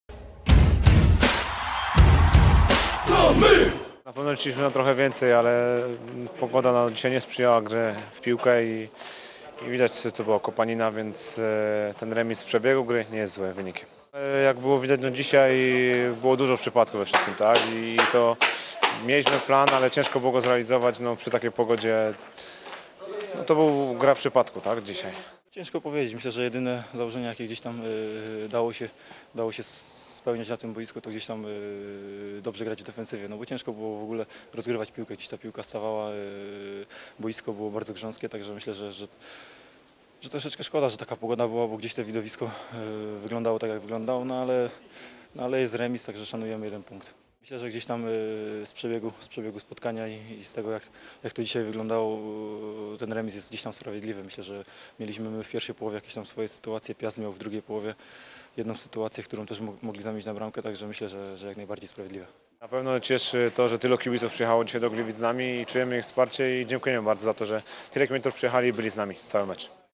Słowik i Matras: Sprawiedliwy remis w trudnych warunkach
Jakub Słowik i Mateusz Matras po remisie z liderem.
Piast Gliwice - Pogoń Szczecin 0:0.